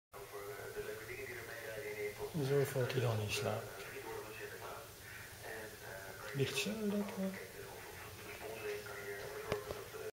lucas slaapt?
zittend slapen, Lucas zit er niet mee. hagazine.wordpress.com